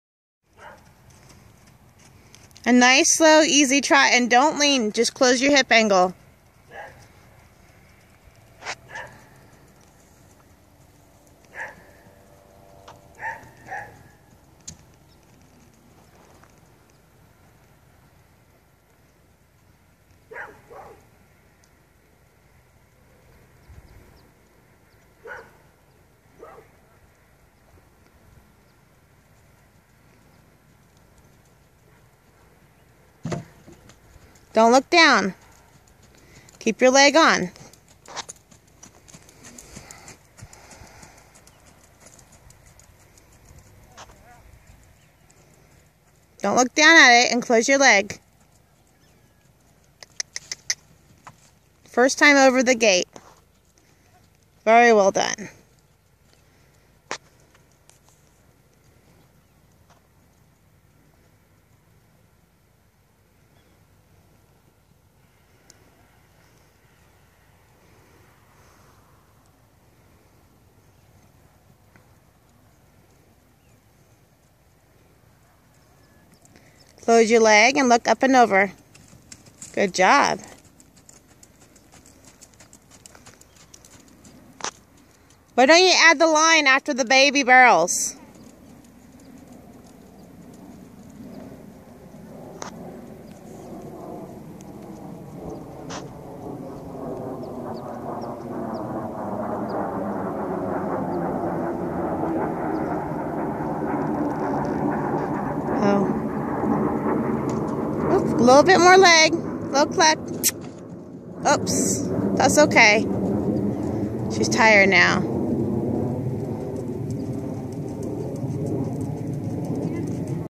Lady Solomae aka Lacy
Jumping a course at 21 days under saddle.